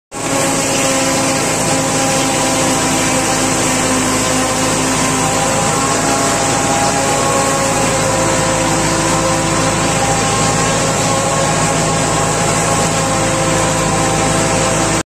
Cortando silagem, milho, sorgo e Mombaça tudo misturado
cortando uma verdadeira salada , milho, Mombaça e sorgo , tudo plantado junto